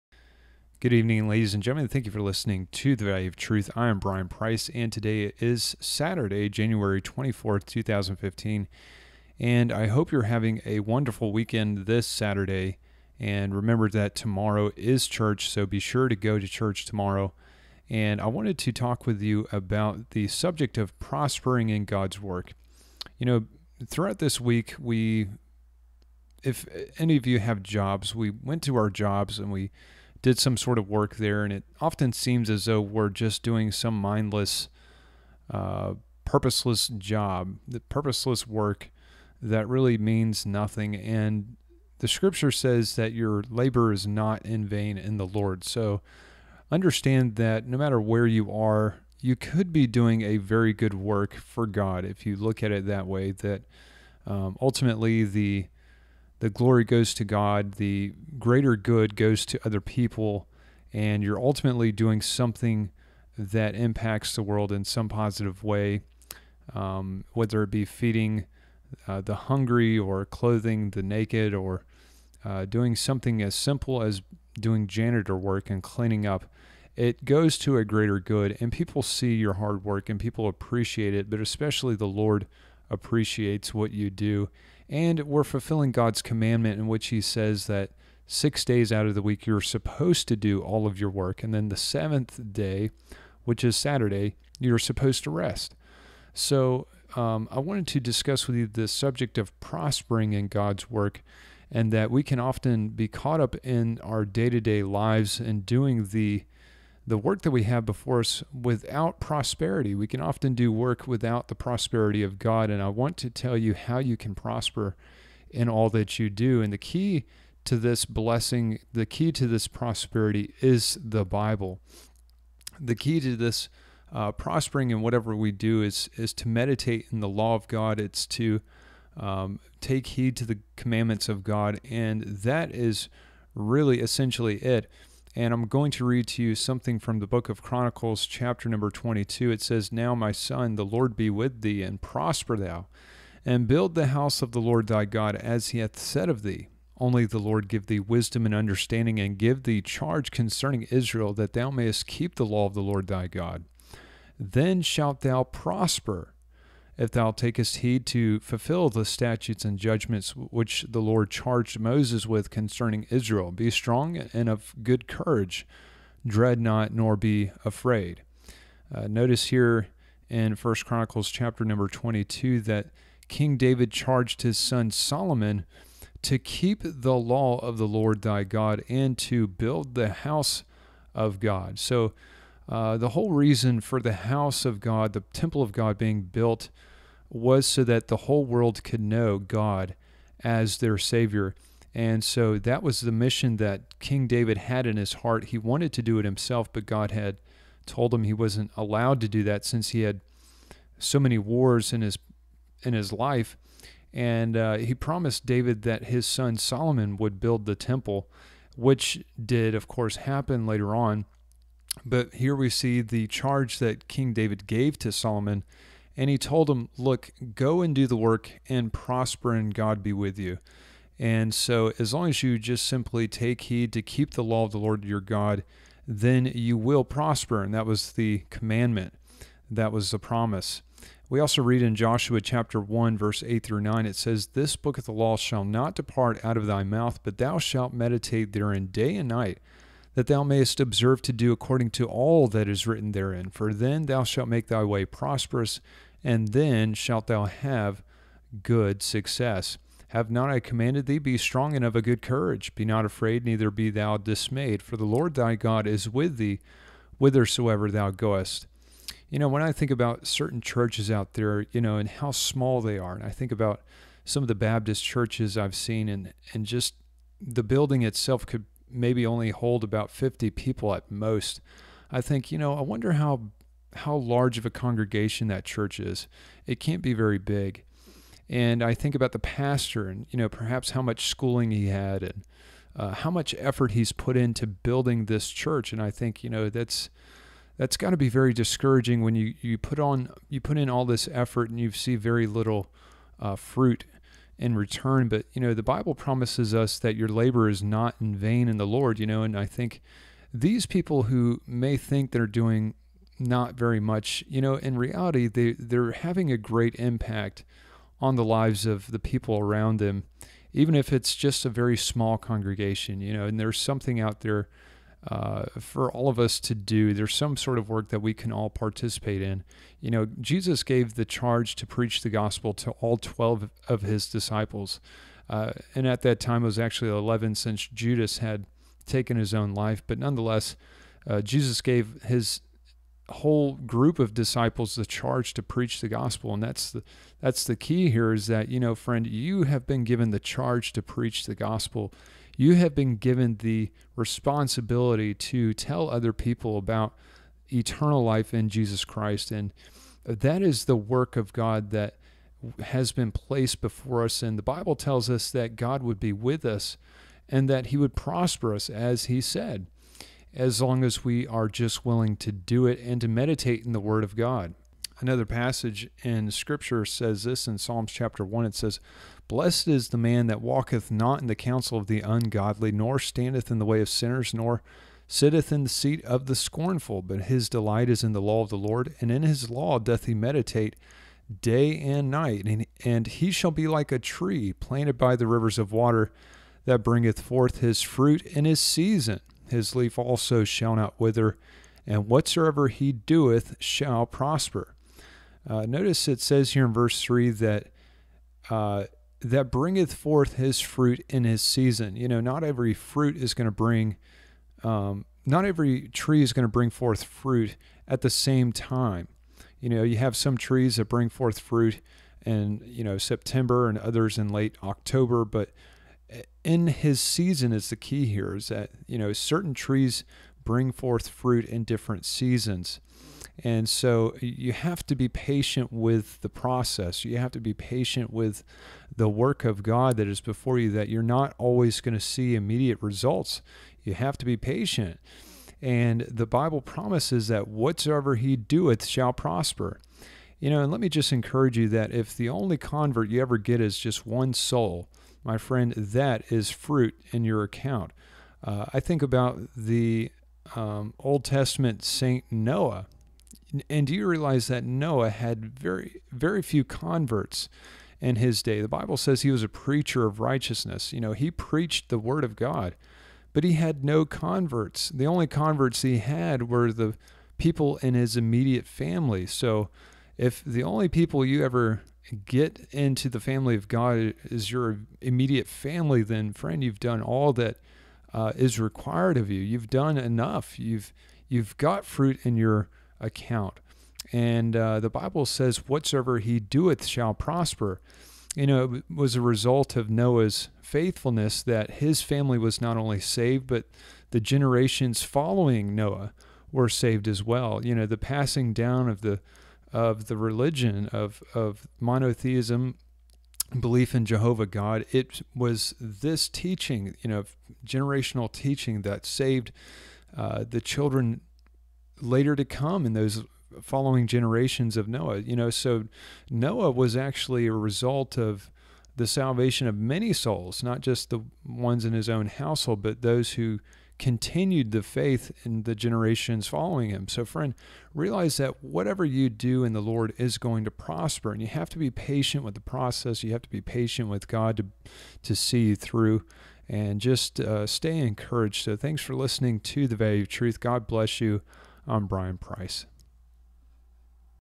Prospering In God's Work
If you died today, would you go to heaven?

Make sure that the work you do is not in vain.  A study from the Bible.